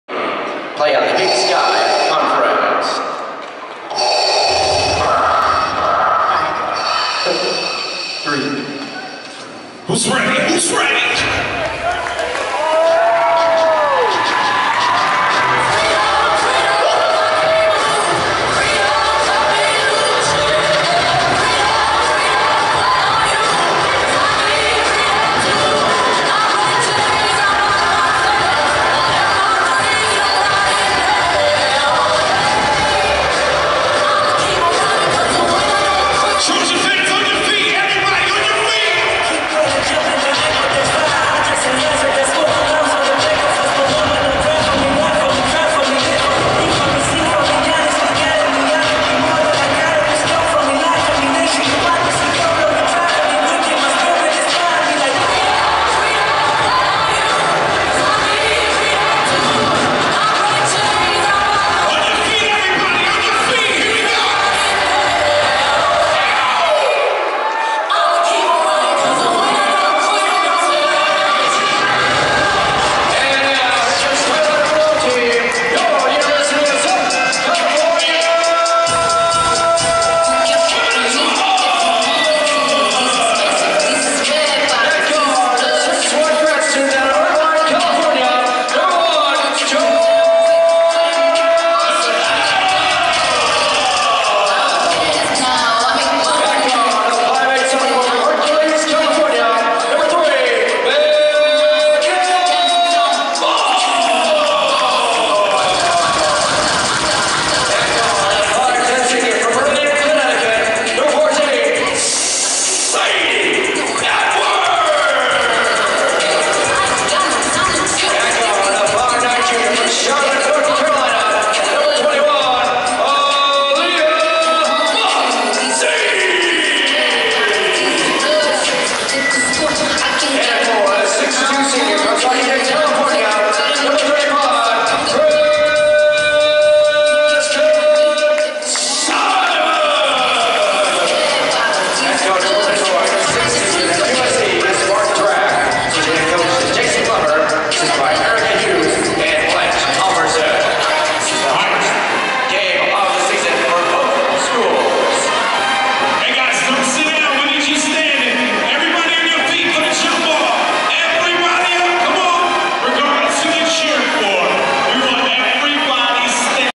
trojancandy.com: Meet the 2017-2018 USC Women's Basketball Starters
At trojancandy.com: The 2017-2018 USC Women of Troy Starters are introduced on the Galen Center big screen.  Read the entire story and see more pictures and videos